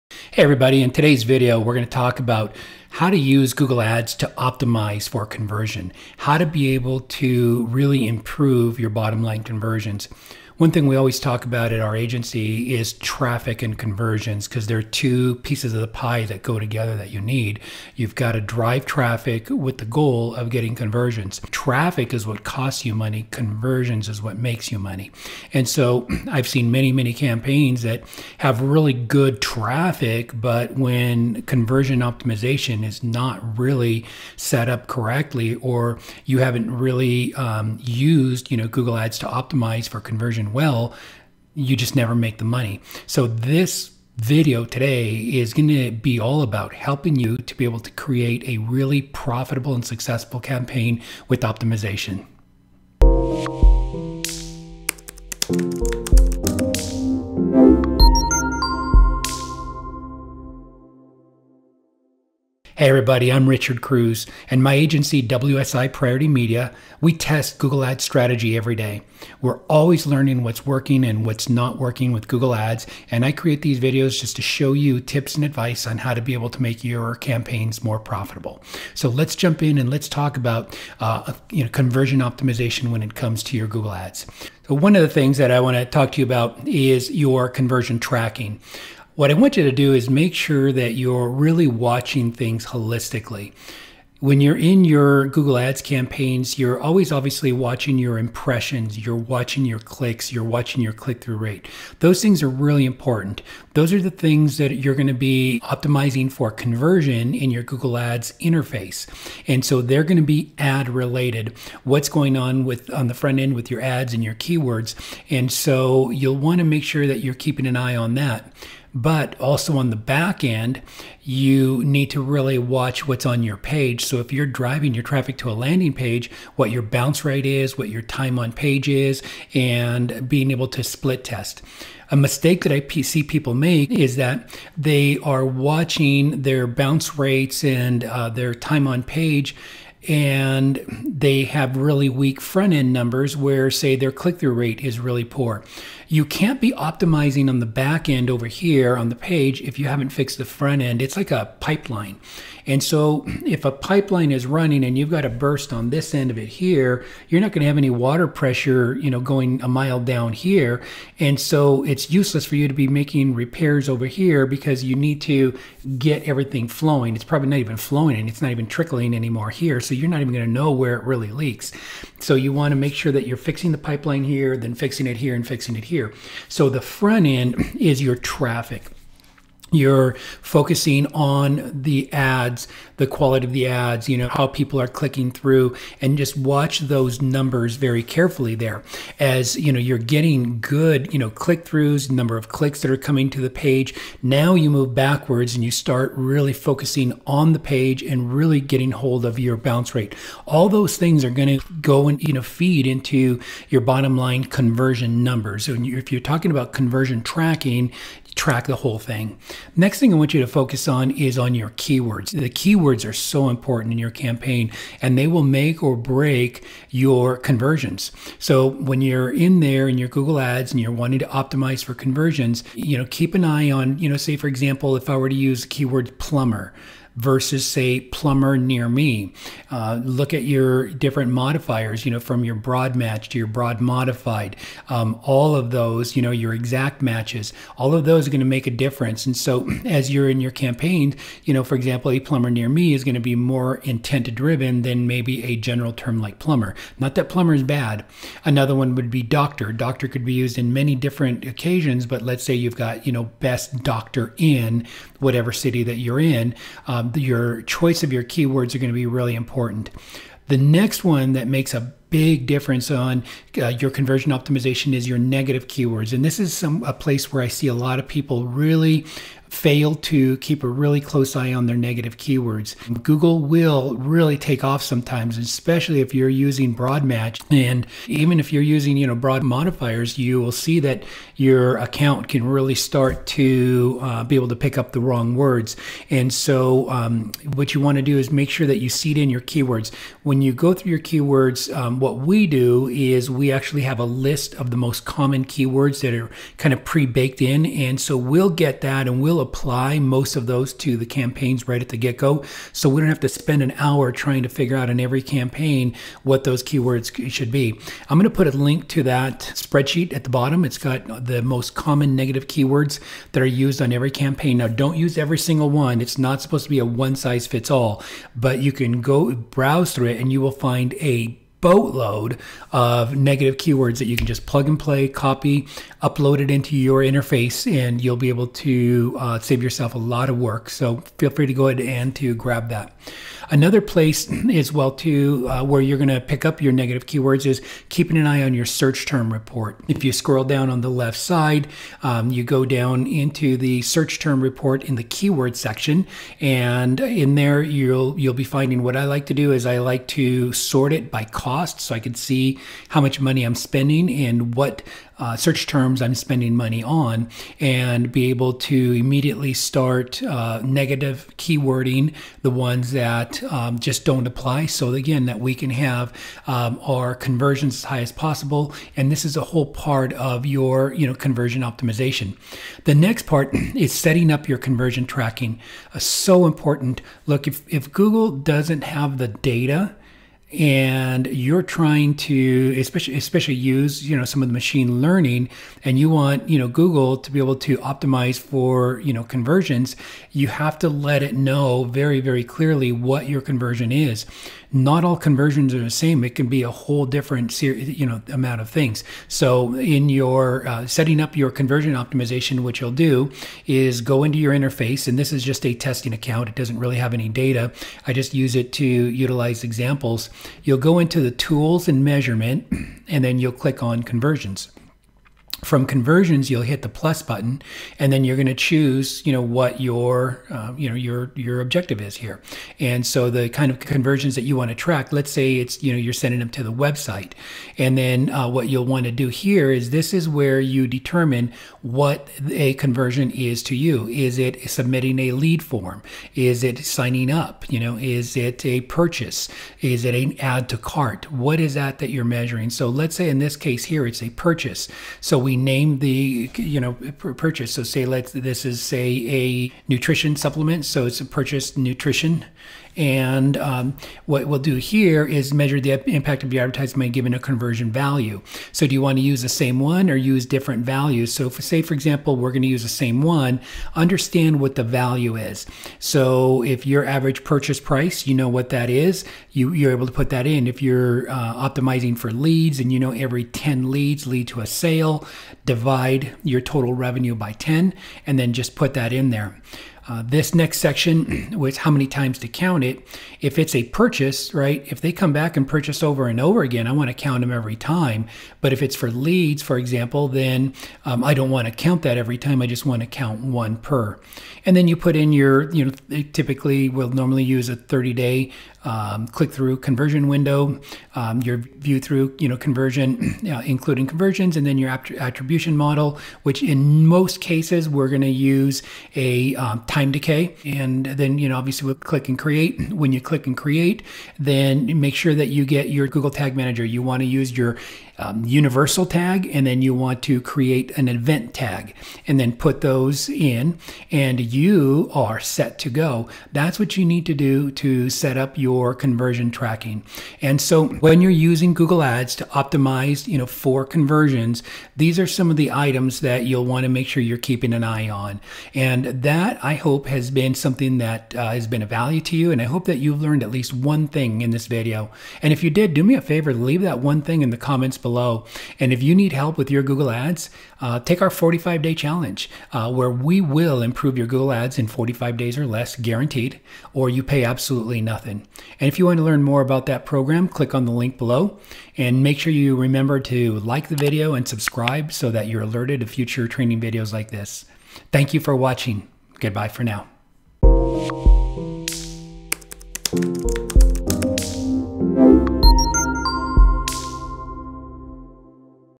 Google Ads Optimize For Conversion Tips + Best Practices
Traffic costs money and conversions make you money. In this video, we discuss how to use Google Ads to Optimize for conversions and make your Google ads campaigns more profitable